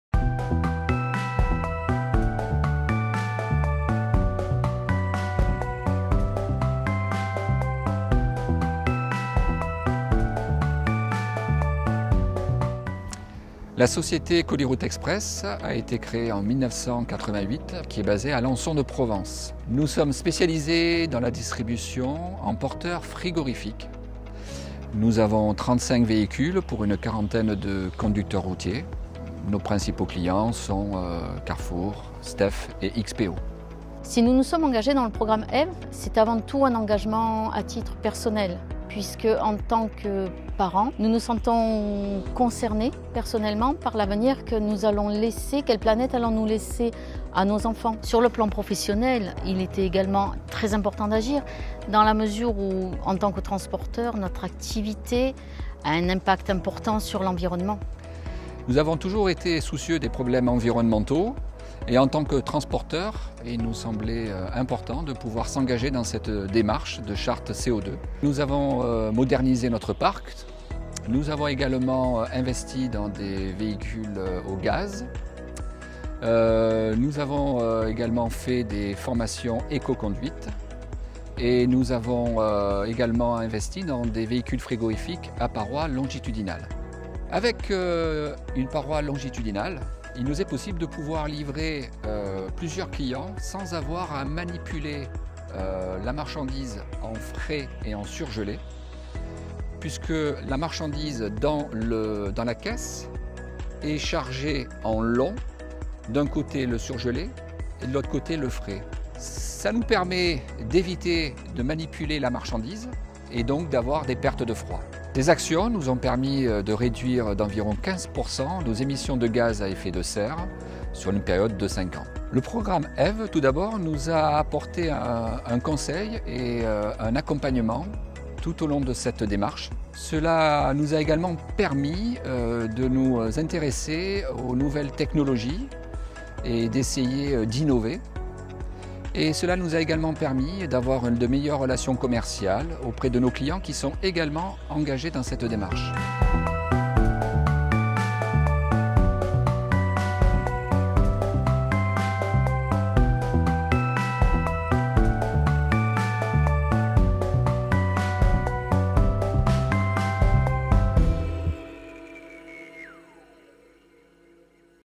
Colis Route Express témoigne : des actions concrètes pour réduire les émissions de GES
Colis Route Express, est un transporteur routier de marchandises, situé en région PACA avec 35 véhicules moteurs. Lauréate des trophées EVE, l'entreprise a diminué de 14,9% ses émissions de GES grâce à plusieurs actions.

Découvrez dans cette vidéo les interventions de Philippe et Nathalie Roth, les gérants.

Les dispositifs du programme EVE soutiennent chaque acteur du secteur dans leur démarche de développement durable : FRET21 pour les chargeurs, EVCom pour les commissionnaires, Objectif CO2 pour les grossistes, transporteurs de marchandises et de voyageurs.